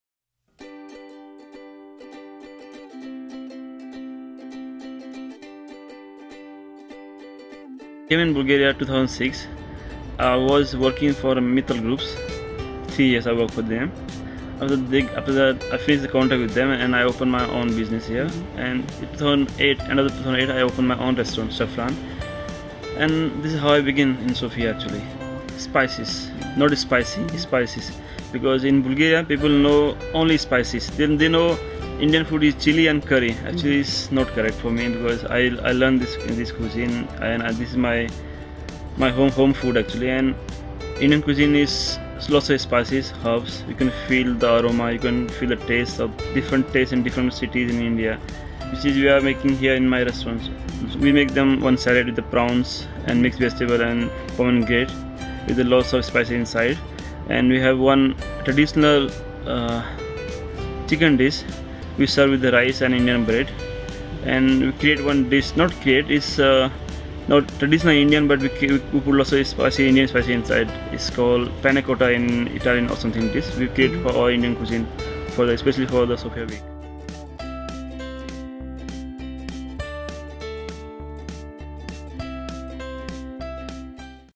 Saffron: Sofia Restaurant Week 2016
Interview with Prakesh owner of Saffron.